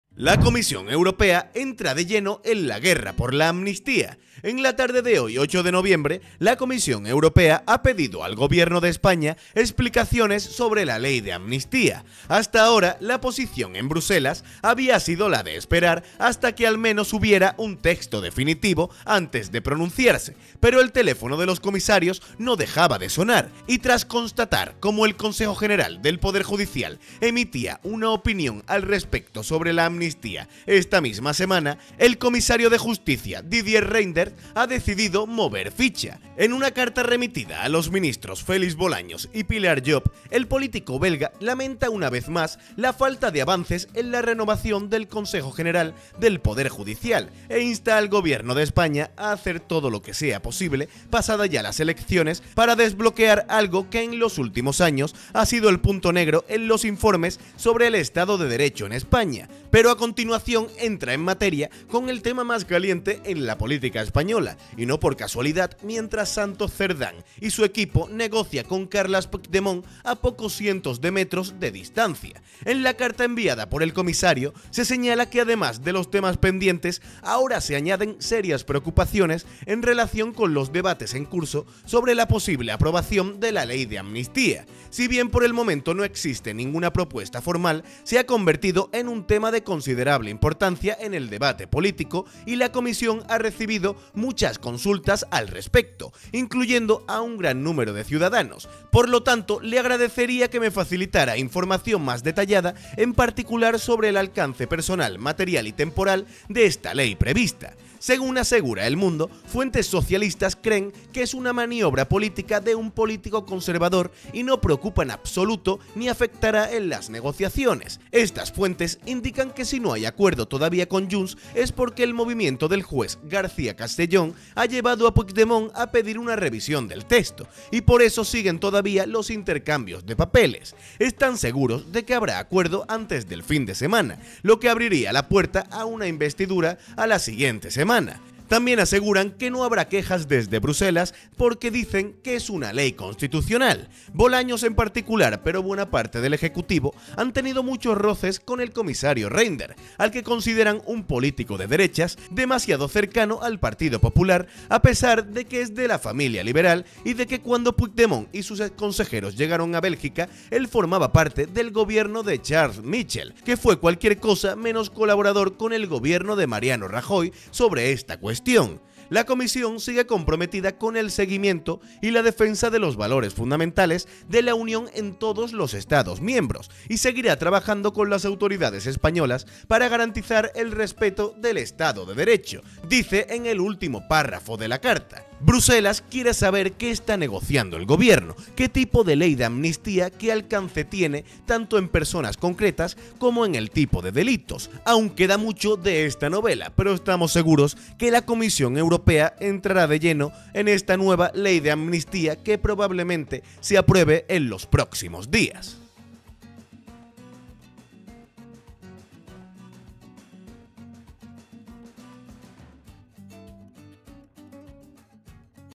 💥¡ACOJONADOS!💥COMISARIO EUROPEO ACOJONA al PSOE CON ESTA CARTA ¡¡POR LA LEY DE AMNISTÍA!!
Didier Reynders acojona a Félix Bolaños y a todo el PSOE con su carta sobre la amnistía.

¿TE HA GUSTADO ESTE VÍDEO? 🔥💥🎙